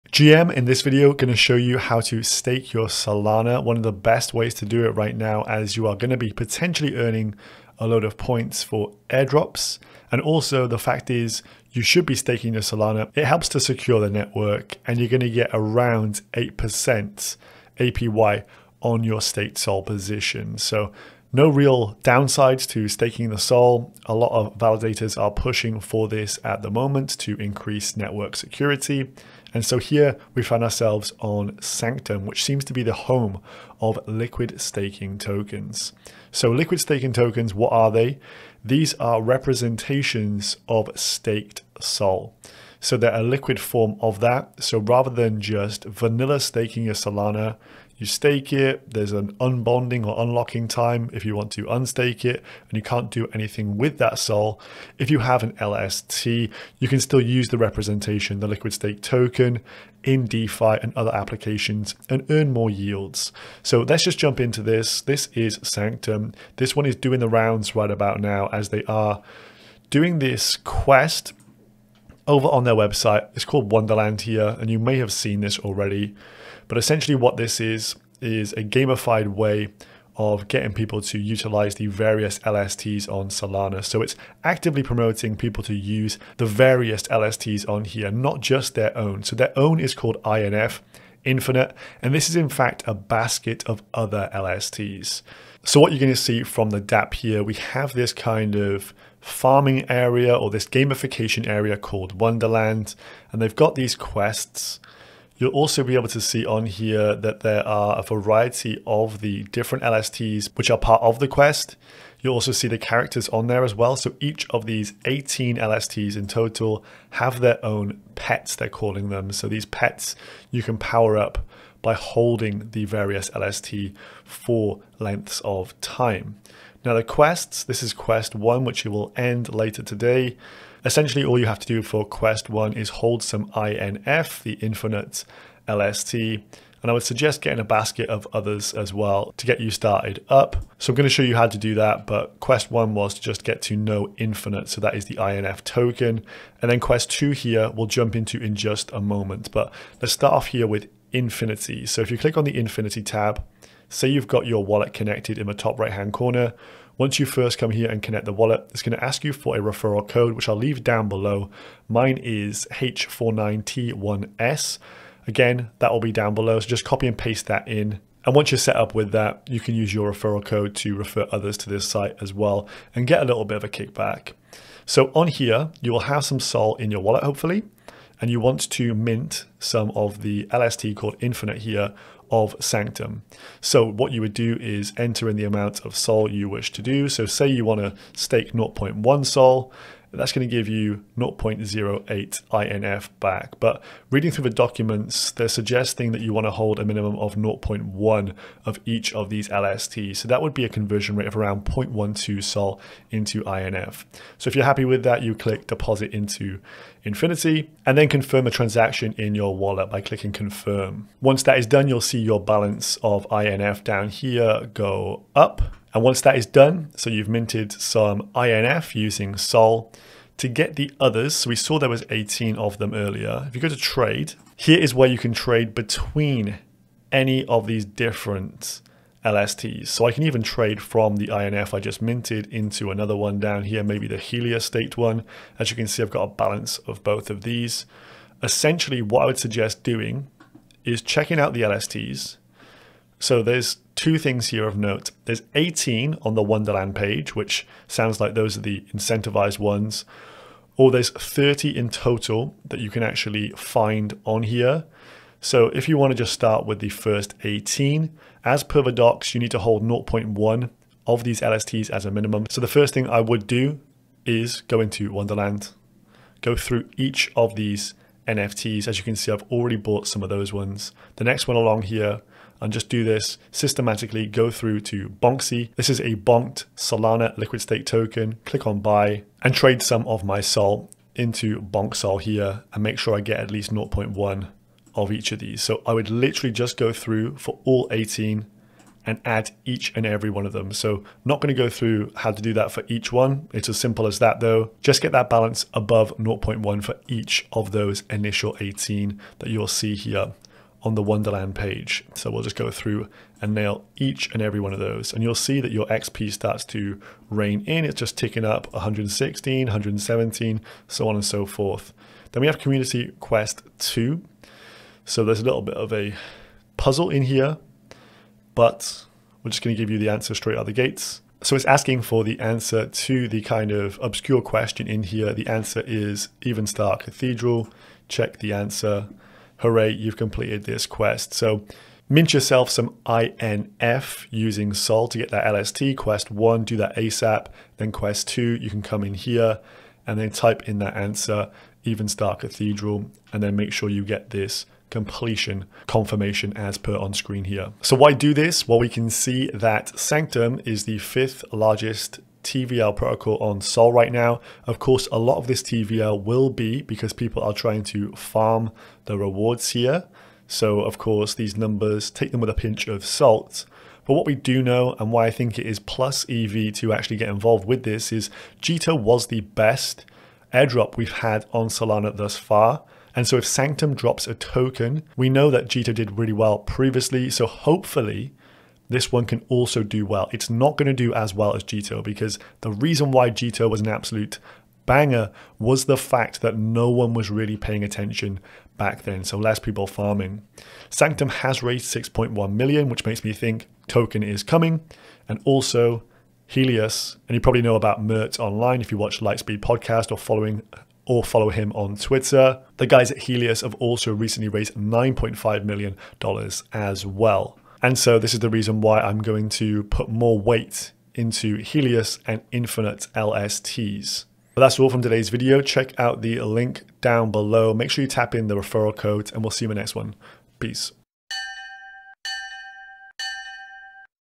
The BEST Way To STAKE SOLANA
In this video we take a look at the best way to stake solana tokens. Sanctum has a great dapp that brings together all the leading Liquid Staking Tokens for Solana under one roof, better yet they are also likely going to drop a token, so Airdrop potential is here.

Quest 2 answer: EVENSTAR CATHEDRAL

*None of this is financial advice

00:00 Staking Solana But Why?
1:05 Sanctum Quests Explained
2:30 How to get INF Liquid Staking Token
3:45 Get all 18 LSTs via Trade Tab
5:10 Quest 2
5:48 Big TVL for LST Protocols
6:30 Sanctum and Helius Tokens?